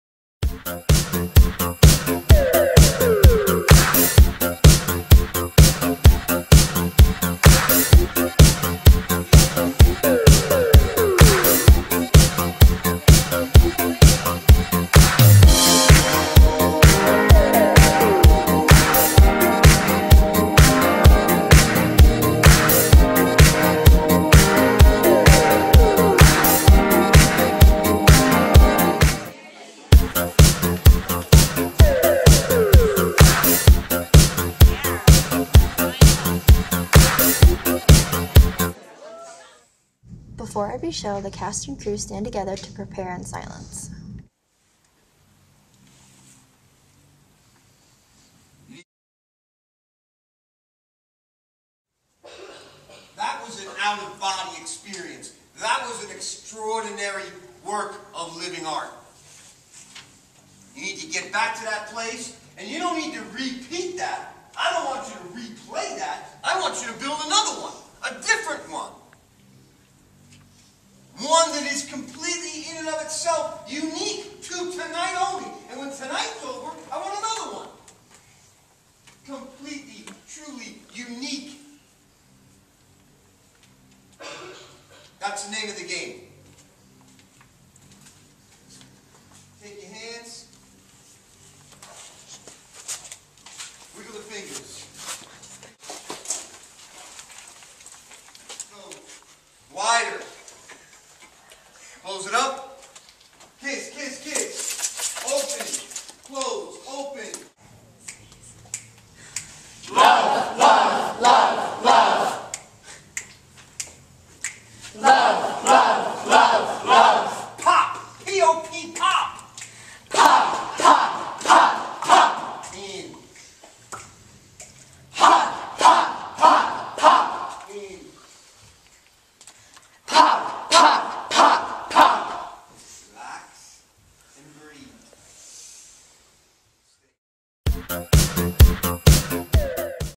Behind the Scenes of WATA's "The Hairy Ape"
Take a behind the scenes look at Westford Academy Theater Arts' production of "The Hairy Ape."